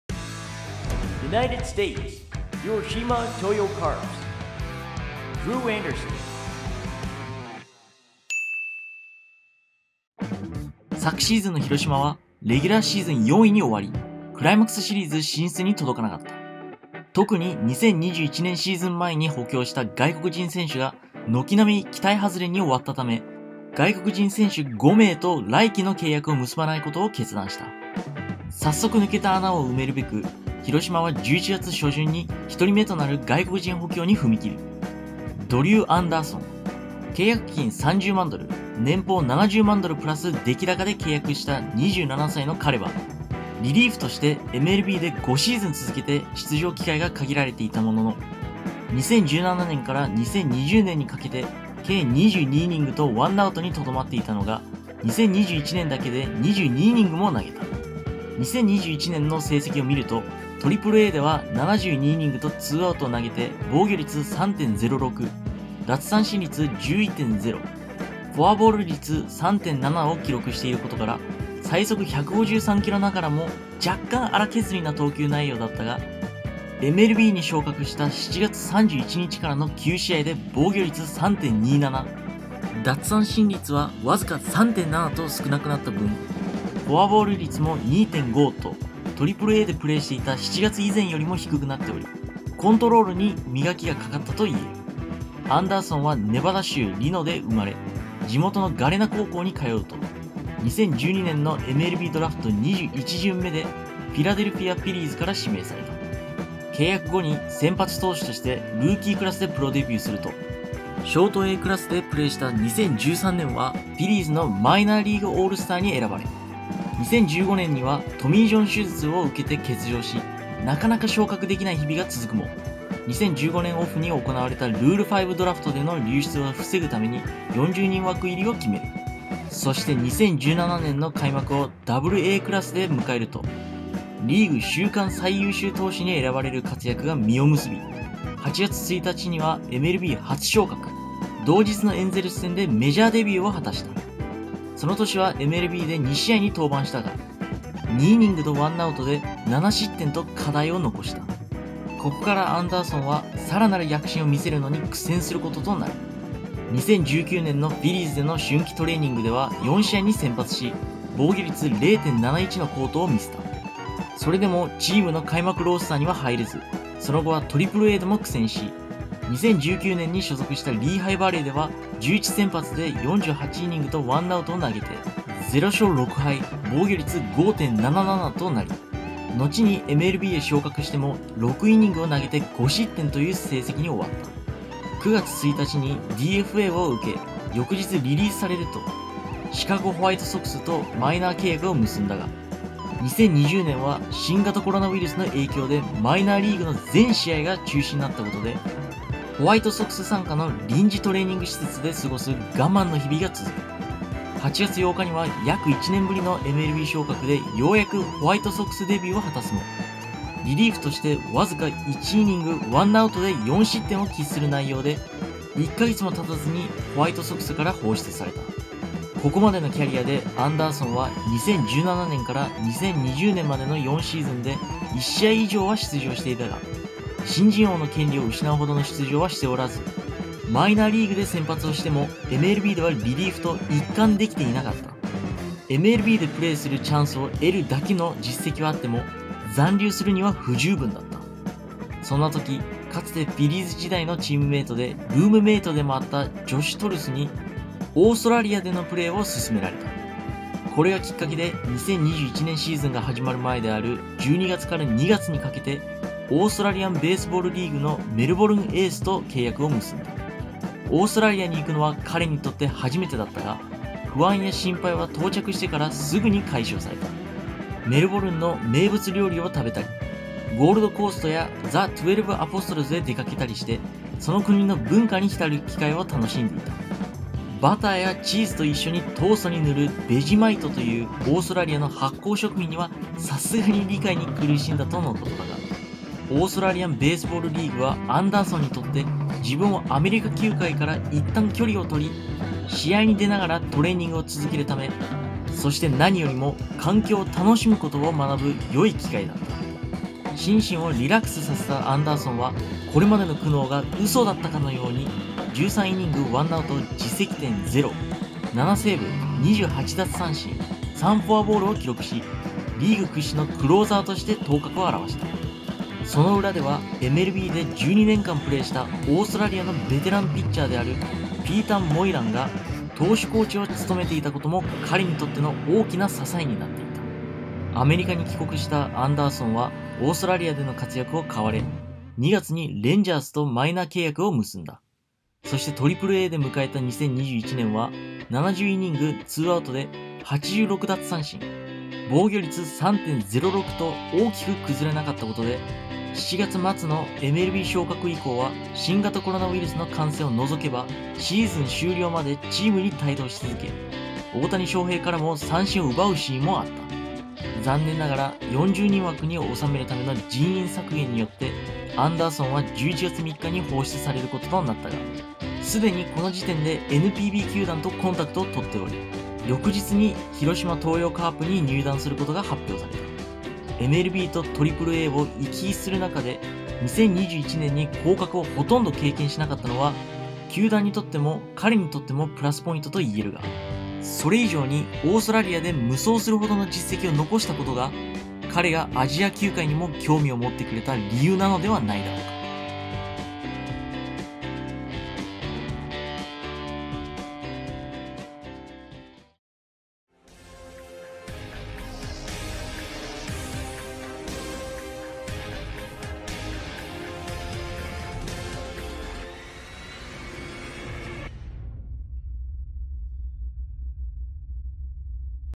【ドリュー・アンダーソン】MLBを目指していたら豪州で最優秀投手に輝いてしまった男
2022年シリーズようやく始動！！
来たる新シーズンに備えるべく、NPBにやってくる外国人選手を紹介していきます。
今回は広島の補強第1号になったドリュー・アンダーソンです！

・Facebookでも更新中！